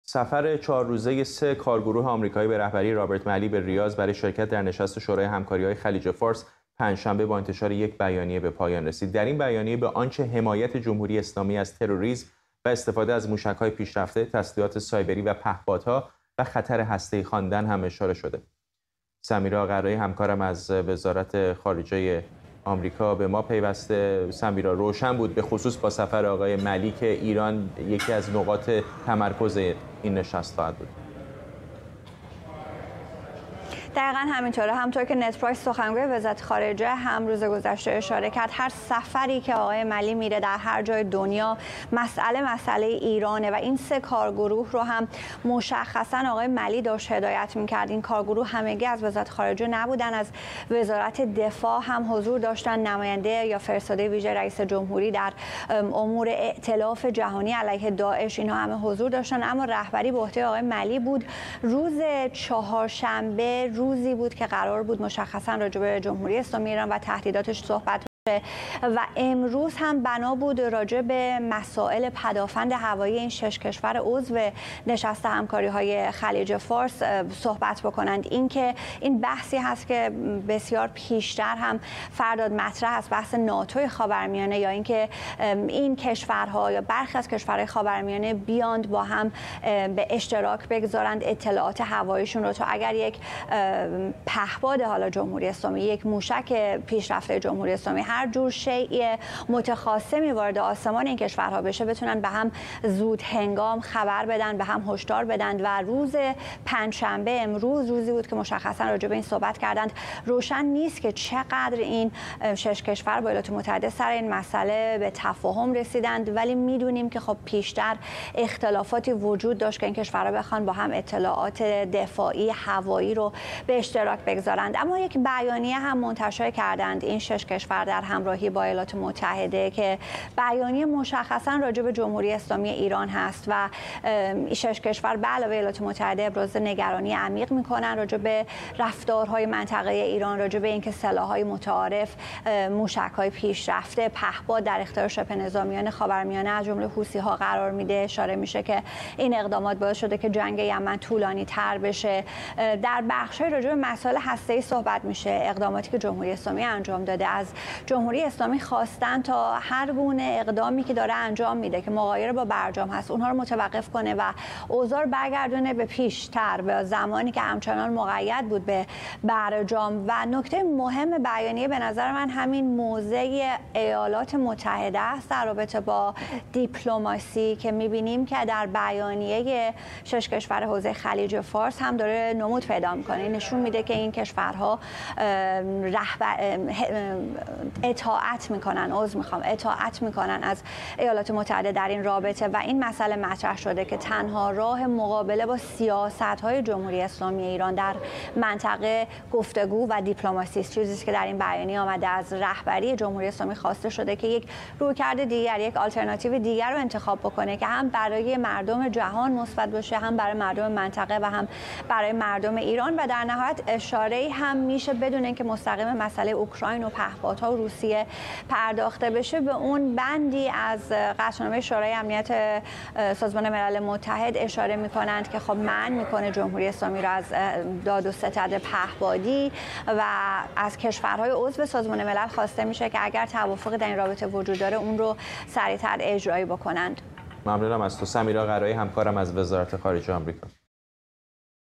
پایان سفر هیئت آمریکایی به ریاض
آمریکا و شورای همکاری در بیانیه‌ای اقدامات بی‌ثابت‌کننده ایران را محکوم کردند؛ این کشورها می‌گویند اقدامات هسته‌ای ایران توجیه صلح‌آمیز ندارند

گزارش سمیرا قرائی، ایران اینترنشنال، از پایان سفر هیئت آمریکایی به ریاض